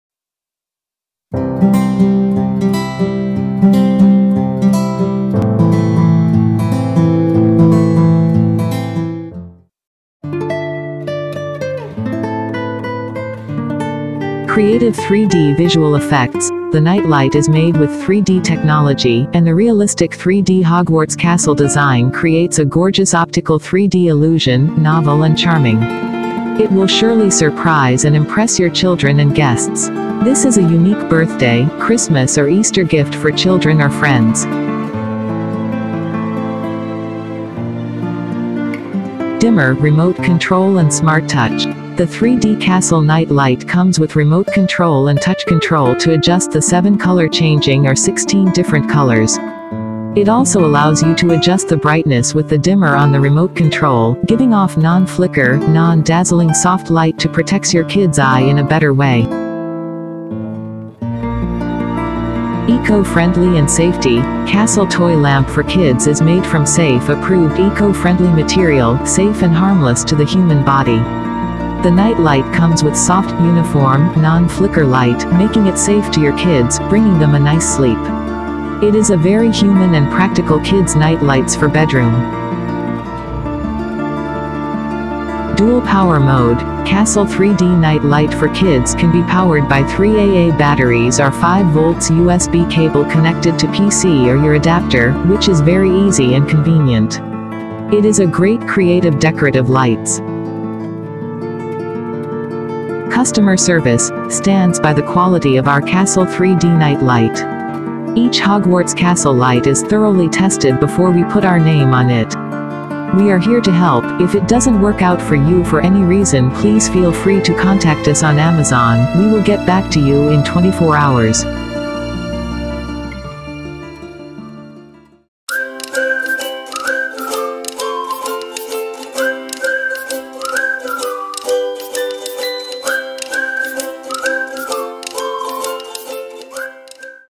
Night Light for Kids Hogwarts Castle LED Décor Lamp with Remote & Smart Touch
Technical Details

Style ‎Art Deco
Brand ‎YOOEI
Product Dimensions ‎3.46"D x 7.3"W x 6.06"H
Special Feature ‎Dimmable
Light Source Type ‎LED
Material ‎Acrylonitrile Butadiene Styrene, Polycarbonate
Shade Material ‎Acrylonitrile Butadiene Styrene, Acrylic
Power Source ‎DC
Controller Type ‎Remote Control
Switch Style ‎Remote
Wattage ‎2.5 watts
Manufacturer ‎Agui-Zhongshan Duoyu
Item Weight ‎6 ounces
Product Dimensions ‎7.3 x 3.46 x 6.06 inches
Batteries ‎1 CR2 batteries required.
Assembled Height ‎6.06 inches
Assembled Length ‎7.3 inches
Assembled Width ‎3.46 inches
Special Features ‎Dimmable